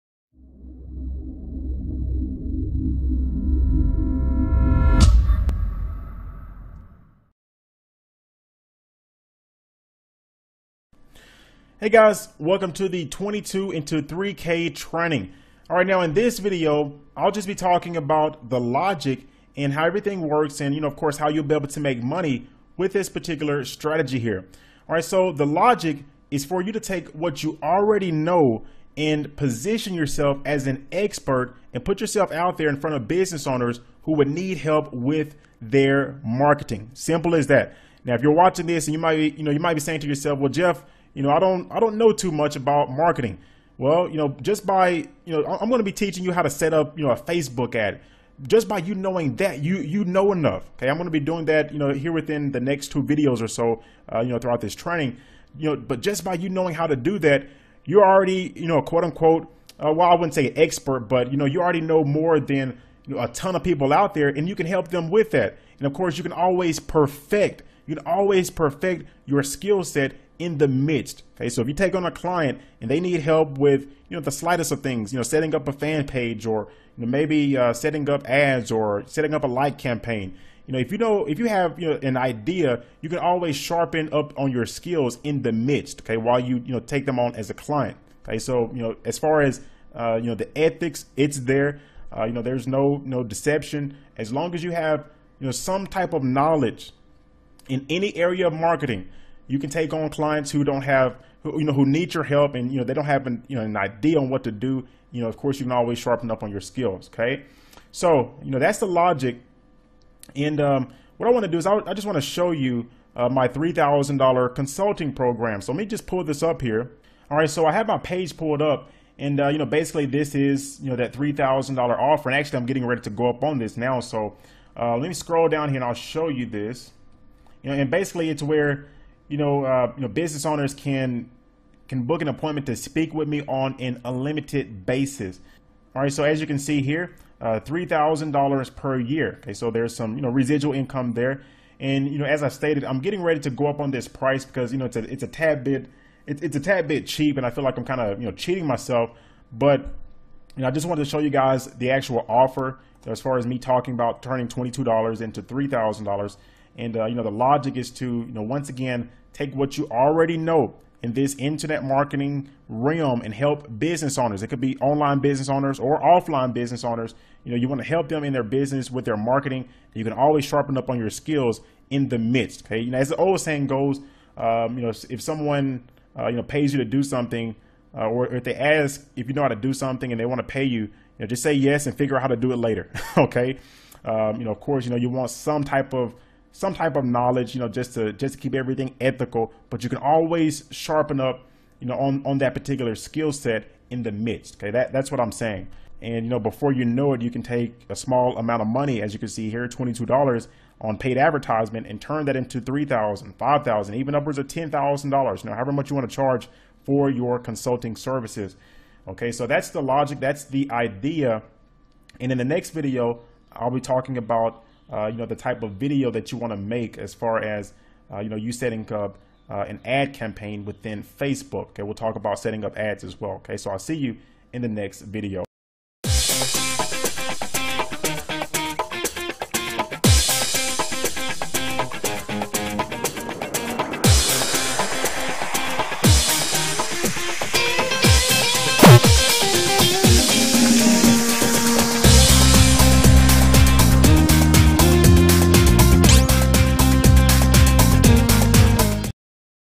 How to turn $22 into $3000 in less than 3 days #1
I am going to show you a method that allowed me to take $22 and turn it into a whopping $3,000 in less than 3 days.

You’ll simply follow these three steps..

Step 1: Find an offer.
Step 2: Implement my secret sauce and traffic campaign.
Step 3: Sit back and make huge profits.